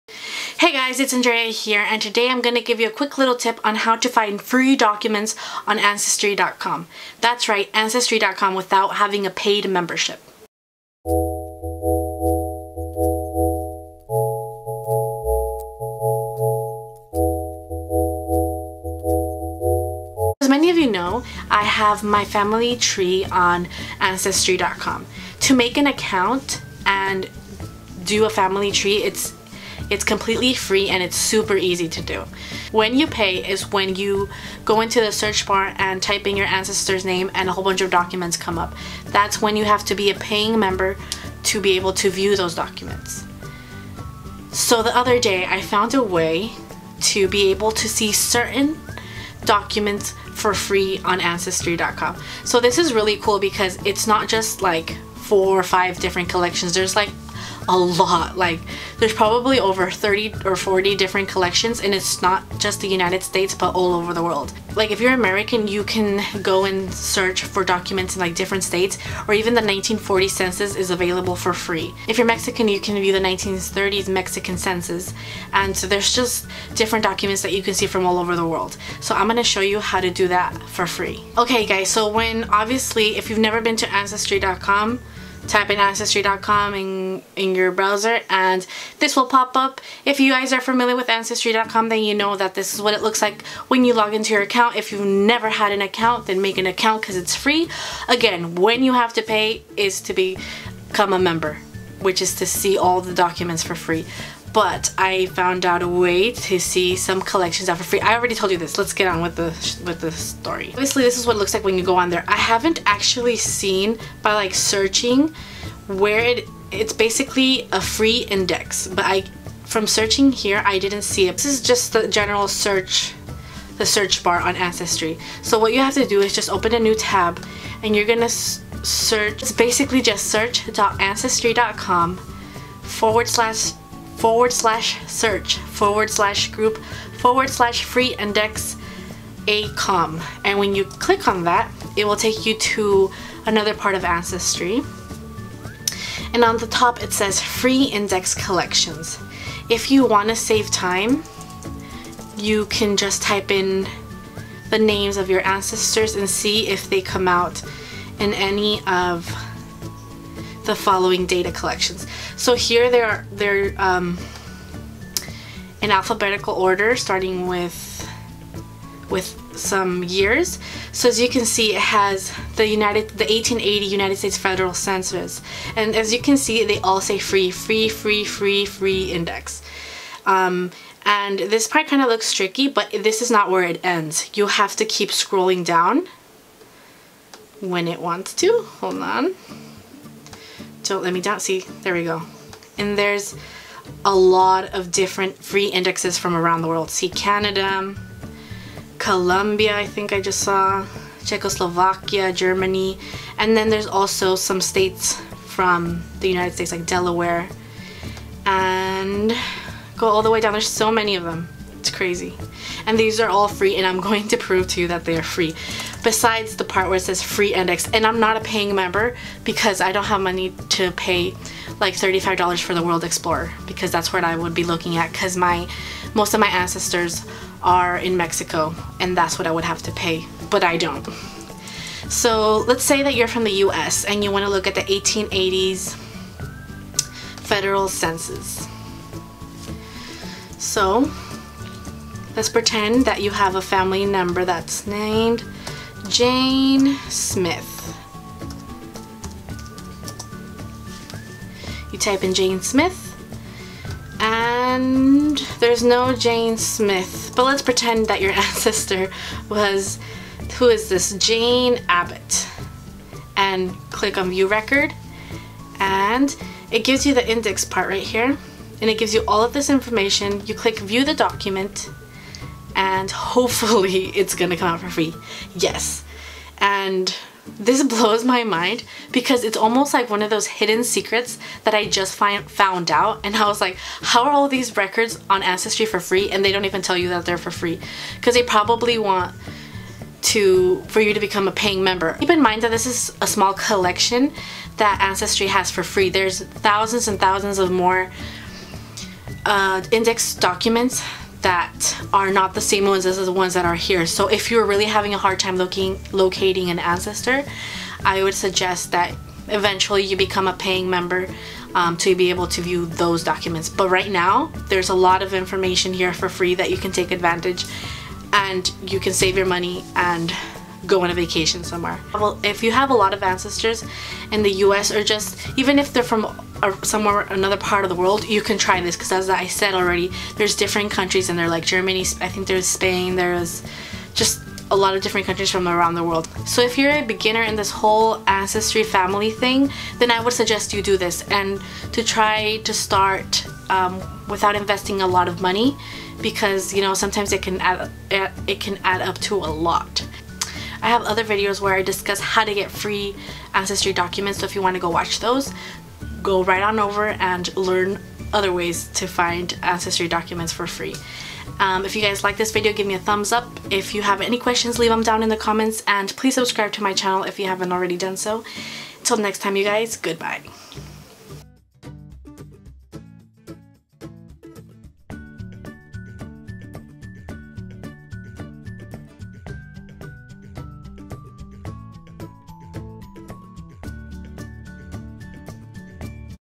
How to view Free Records on Ancestry.com | Free Index Ancestry Documents, Records, Research
Find out how to access the free index collections of Ancestry Documents on Ancestry.com completely free!

Keep in mind to view the regular collections you do have to have a paid membership. But making an account and building a family tree is completely free!

Leave any questions regarding ancestry research below.